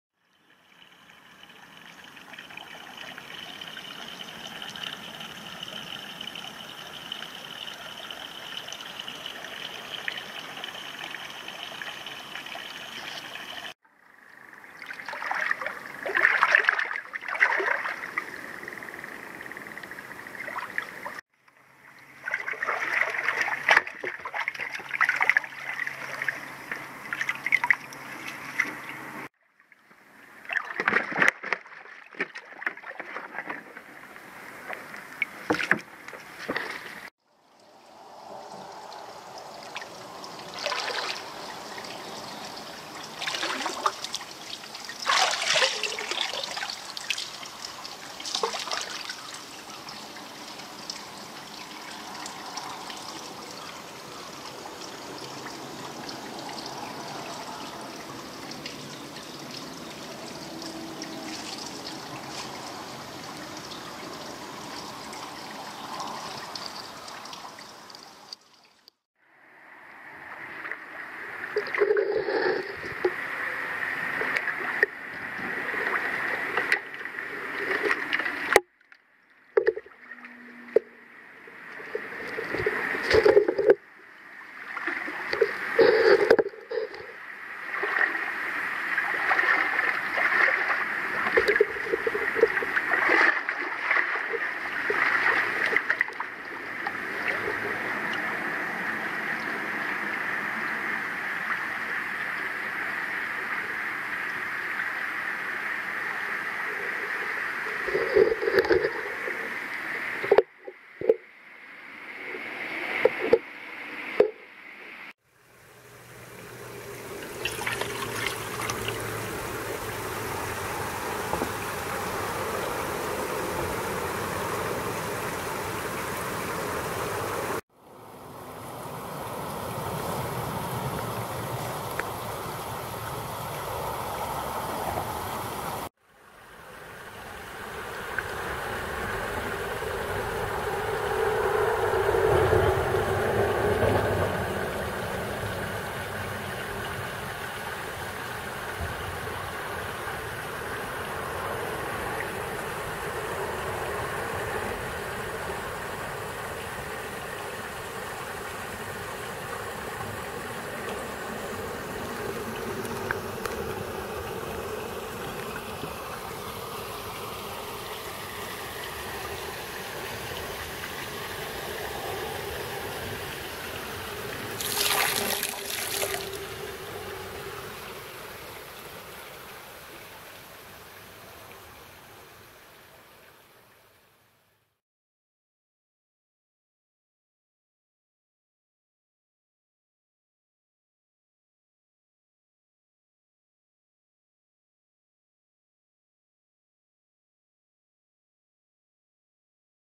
農業用水でガサガサ。背びれに黒い斑点、ここでは見慣れない魚だけど…
ナマズがたくさん棲んでいる農業用水でガサガサ。背びれに黒い斑点、ここでは見慣れない魚が捕れた。何だろう？